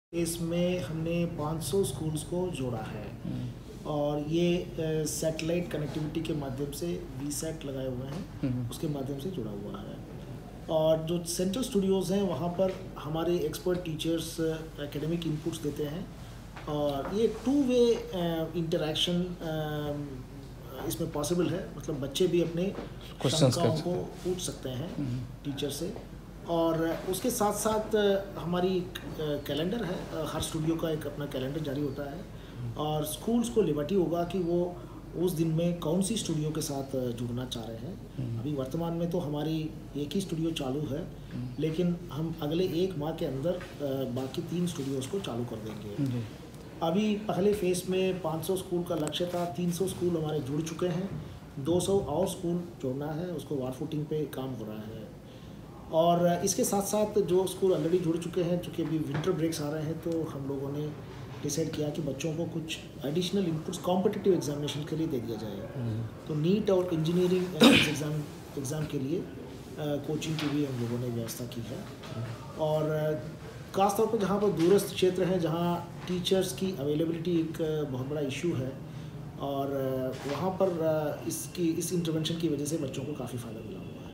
स्मार्ट क्लास उत्तराखंड| Smart Class Uttarakhand| IAS Meenkashi Sundaram
प्रदेश के सरकारी स्कूलों में जहां एक तरफ स्मार्ट क्लास को लेकर राज्य सरकार  स्कूलों में शिक्षकों की कमी दूर करने का रास्ता निकाल रही है वहीँ दूसरी ओर शिक्षा विभाग ने एक नै पहला शुरु की है.दरअसल सर्दियों की छुट्टियों में शिक्षा विभाग स्मार्ट वर्चुअल क्लास के जरिये स्कूली बच्चों को कंपटीशन एग्जाम की कोचिंग देने जा रहा है. इसमें इंजीनियरिंग के अलावा NEET मेडिकल और कई मनेजमेंट कोरेसेस की तैयारी करवाई जाएगी. इतना ही नहीं आगामी प्रतियोगी परीक्षा को लेकर भी छात्रों को टिप्स दिए जाएंगे. शिक्षा सचिव मीनाक्षी सुंदरम ने इस बाबत जानकारी देते हुए कहा की वर्तमान में स्मार्ट क्लासेस के जरिए 300 स्कूलों में डारेक्ट बच्चों को जोड़ा जा रहा है. फ़िलहाल देहरादून में एक डिजिटल स्टूडियो है जबकि अब तीन और डिजिटल स्टूडियो की शुरुआत इसी महीने में हो जाएगी. जिसके बाद पहले चरण की सभी 500 स्कूलों तक राज्य सरकार स्मार्ट क्लासेस के जरिए पहुंचेगी. सचिव मीनाक्षी सुंदरम ने यह भी बताया कि स्कूली बच्चों को कंपटीशन एग्जाम के लिए तैयार किया जाएगा ताकि सर्दियों की छुट्टियों में बच्चे अलग सब्जेक्ट की कुछ तैयारी करना चाहे, तो वह फायदा उठा सकें.